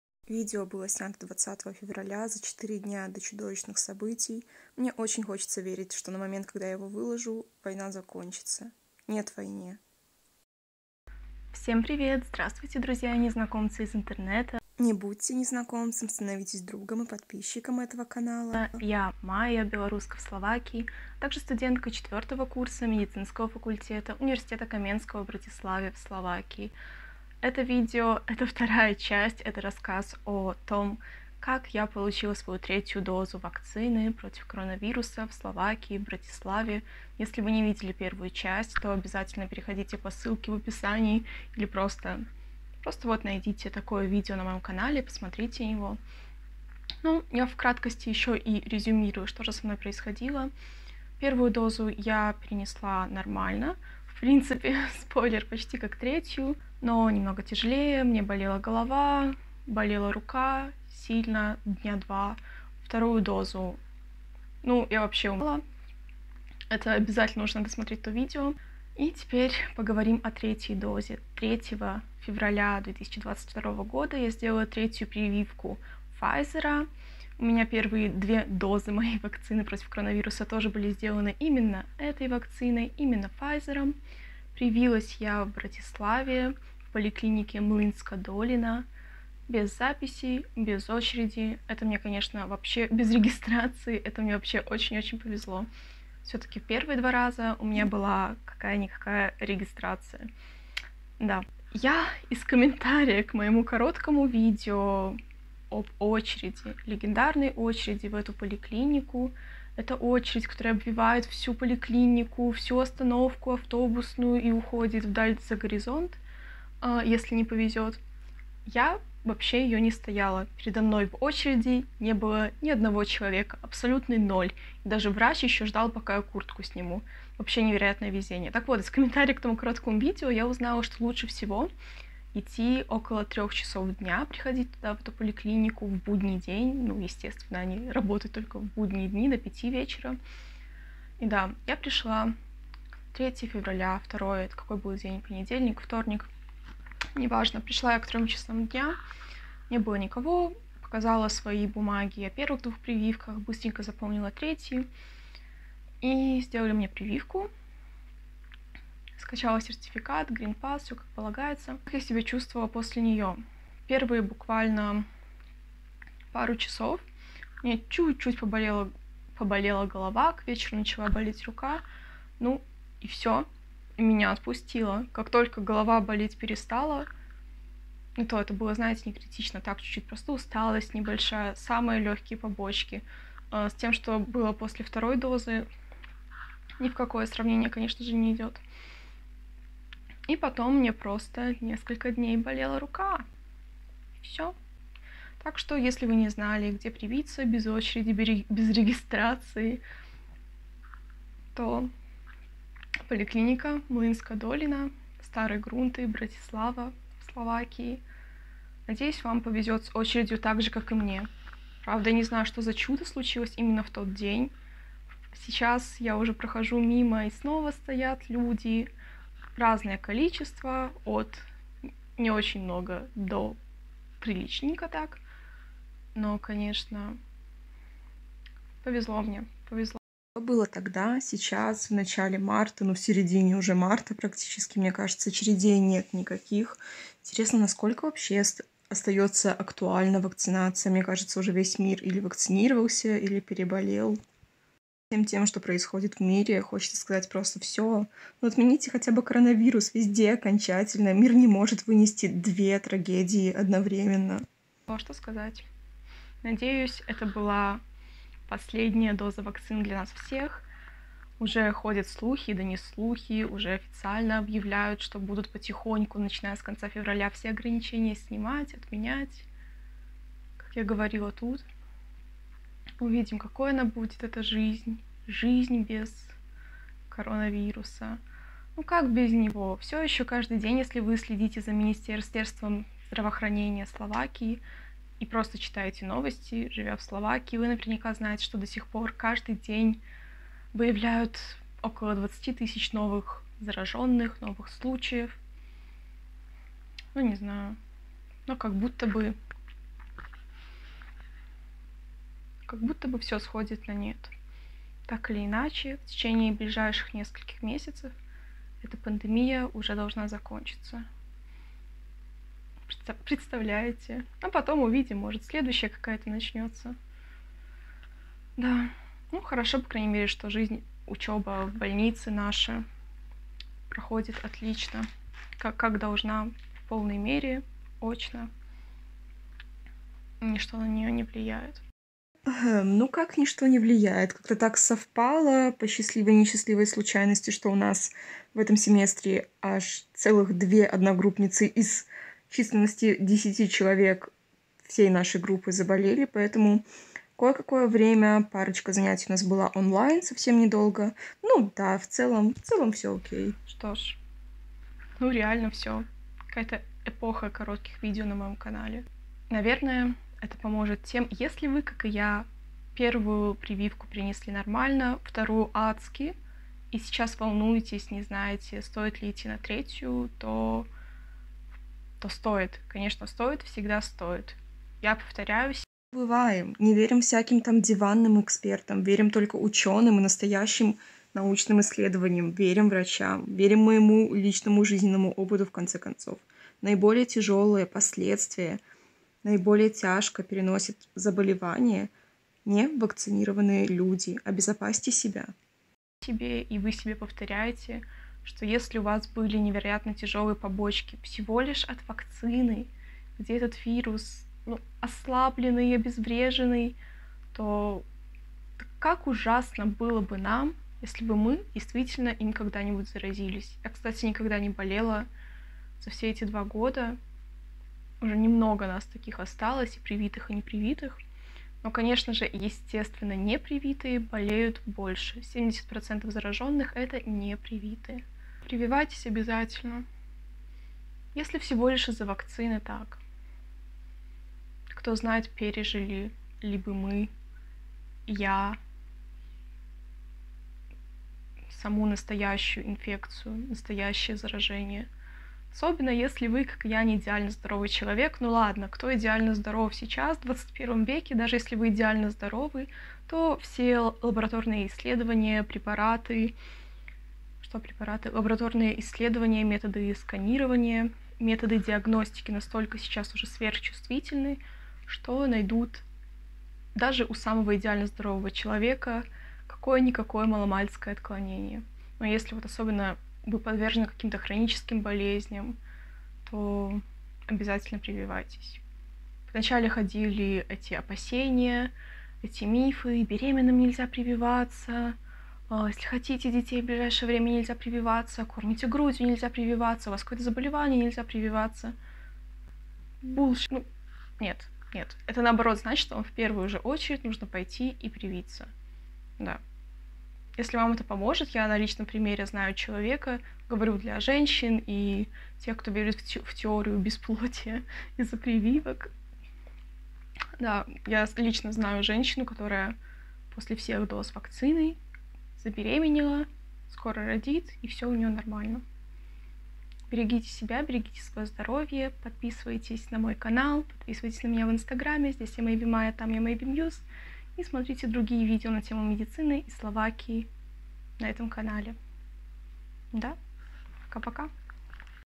ЖИЗНЬ В СЛОВАКИИ коронавирус третья доза вакцины COVID-19| конец пандемии? что нужно знать правда 🇸🇰
жизньвсловакии коронавирусвсловакии вакцинациявсловакии Добрый день, сегодня поговорим о третьей прививке, третьей дозе вакцины от COVID-19 в Словакии...